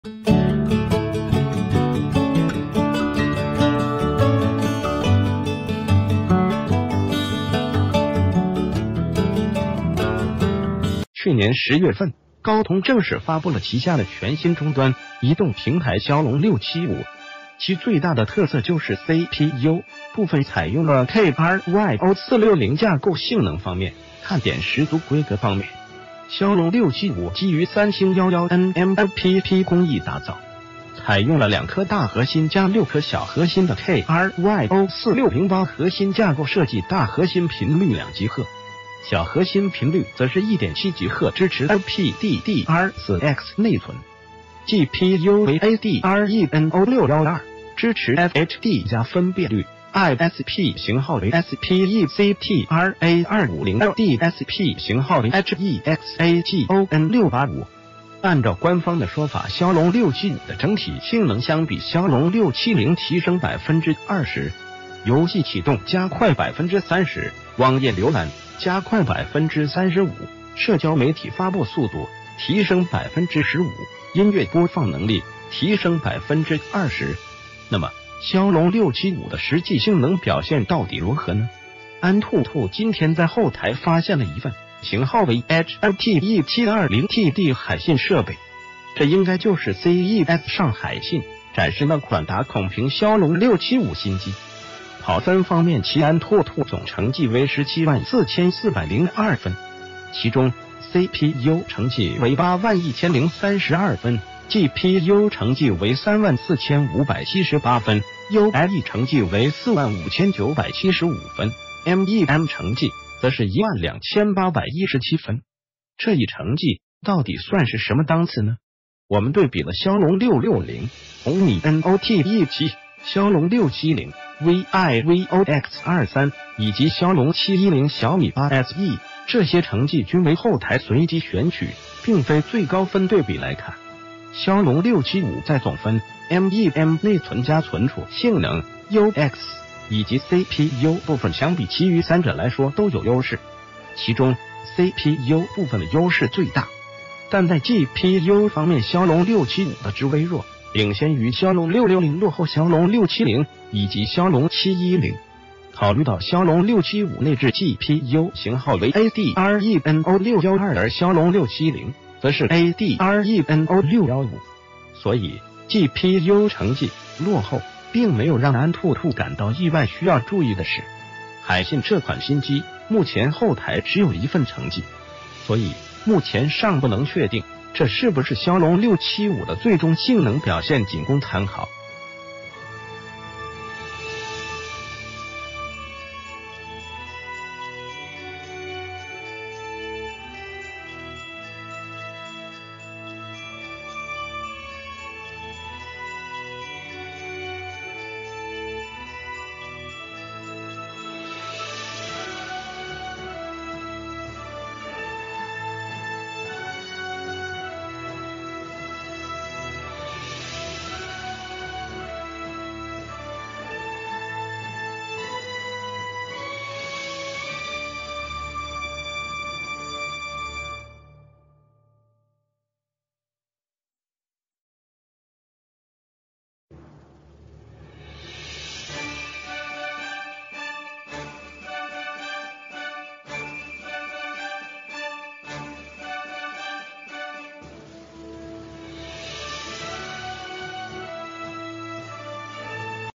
驍龍675安兔兔跑分曝光：傲視驍龍660/670/710
谢谢！

    去年10月份，高通正式發布了旗下的全新中端移動平臺驍龍675，其最大的特色就是CPU部分采用了Kryo 460架構，性能方面看點十足。
    規格方面，驍龍675基于三星11nm LPP工藝打造，采用了兩顆大核心+六顆小核心的Kryo 460八核心架構設計，大核心頻率2GHz，小核心頻率則是1.7GHz，支持LPDDR4X內存，GPU為Adreno 612（支持FHD+分辨率），ISP型號為Spectra 250L，DSP型號為Hexagon 685。
    按照官方的說法，驍龍675的整體性能相比驍龍670提升20%，游戲啟動加快30%、網頁瀏覽加快35%、社交媒體發布速度提升15%、音樂播放能力提升20%。
    那么，驍龍675的實際性能表現到底如何呢？安兔兔今天在后臺發現了一份型號為“HLTE720T”的海信設備，這應該就是CES上海信展示那款打孔屏驍龍675新機。
    跑分方面，其安兔兔總成績為174402分，其中CPU成績為81032分、GPU成績為34578分、UX成績為45975分、MEM成績則是12817分。
    這一成績到底算是什么檔次呢？我們對比了驍龍660（紅米Note 7）、驍龍670（vivo X23）以及驍龍710（小米8 SE），這些成績均為后臺隨機選取，并非最高分。
    對比來看，驍龍675在總分、MEM（內存+存儲）性能、UX以及CPU部分相比其余三者來說都有優勢，其中CPU部分的優勢最大。
    但在GPU方面，驍龍675的只微弱領先于驍龍660，落后驍龍670以及驍龍710。考慮到驍龍675內置GPU型號為Adreno 612，而驍龍670則是Adreno 615，所以GPU成績落后并沒有讓安兔兔感到意外。
    需要注意的是，海信這款新機目前后臺只有一份成績，所以目前尚不能確定這是不是驍龍675的最終性能表現，僅供參考。如有更多成績，安兔兔會及時分享給大家。